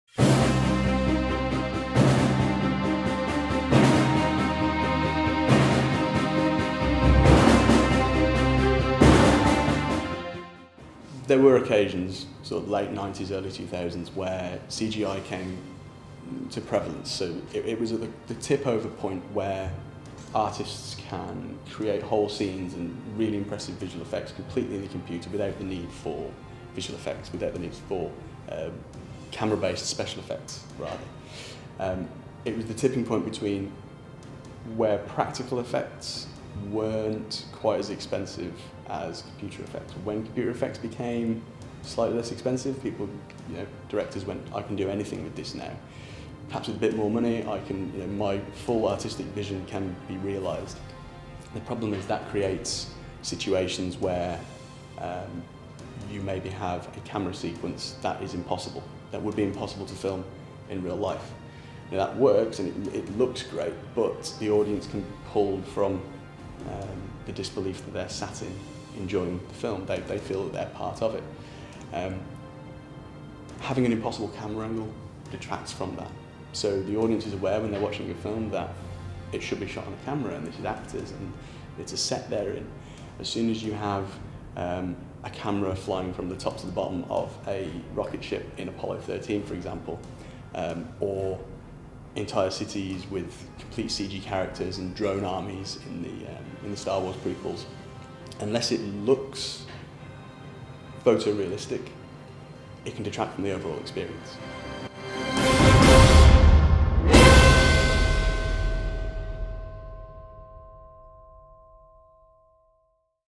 Episode II: Practical effects vs CGI in Star Wars
Dr Anthony Bukowski, Senior Lecturer in Computer Animation & VFX,  talks about the use of CGI in the Star Wars franchise.